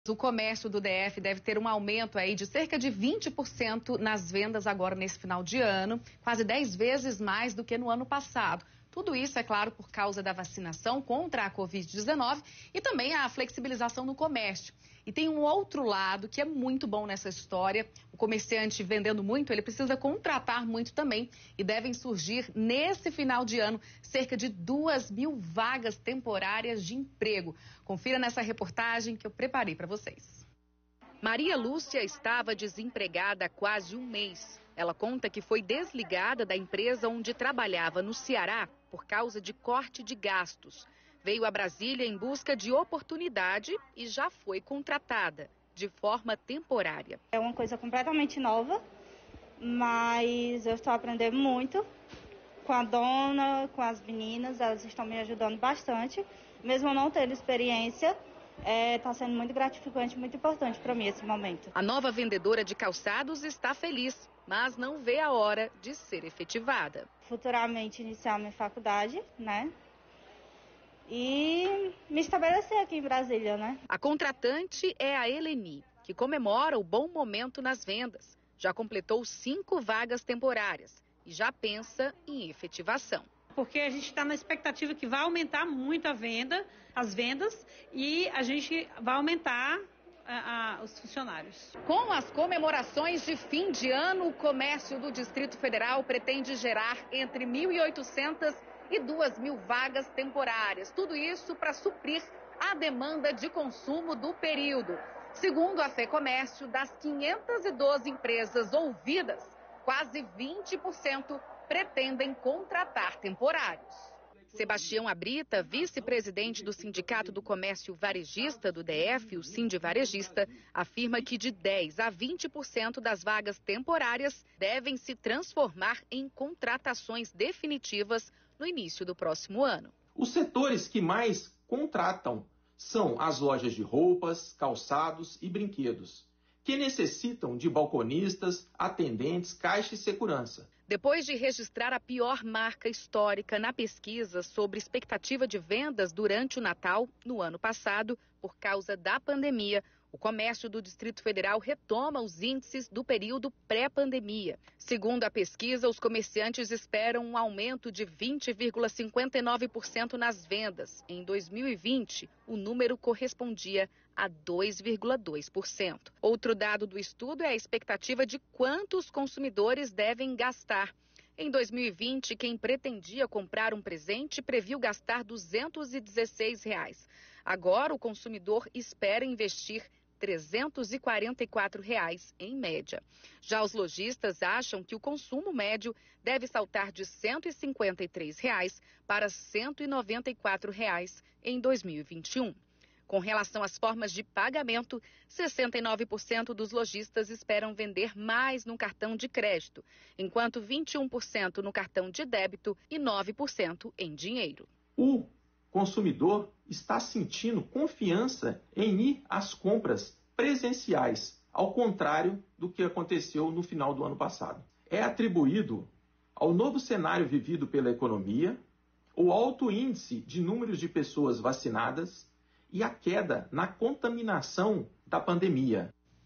Comércio prevê aumento de 20% nas vendas de final de ano
O comércio do Distrito Federal prevê um aumento de 20% nas vendas de final do ano. Além disso, para os últimos meses do ano, estão previstas ainda a abertura de cerca de dois mil empregos temporários.